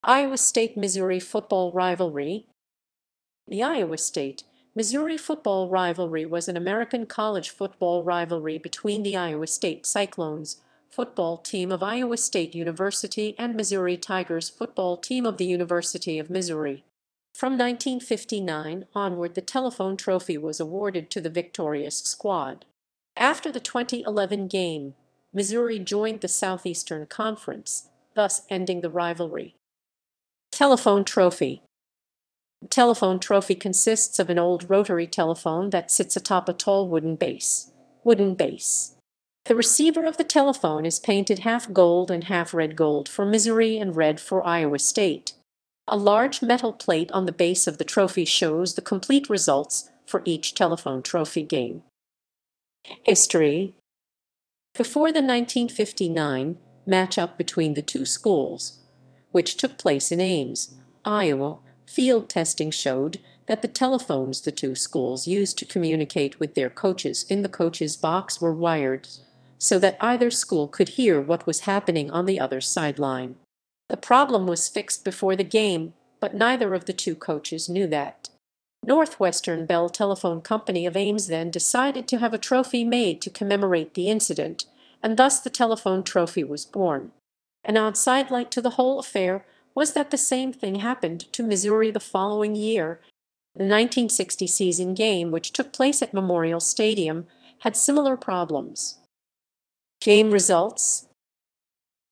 Iowa State–Missouri football rivalry
The Iowa State–Missouri football rivalry was an American college football rivalry between the Iowa State Cyclones football team of Iowa State University and Missouri Tigers football team of the University of Missouri. From 1959 onward the Telephone Trophy was awarded to the victorious squad.After the 2011 game, Missouri joined the Southeastern Conference, thus ending the rivalry.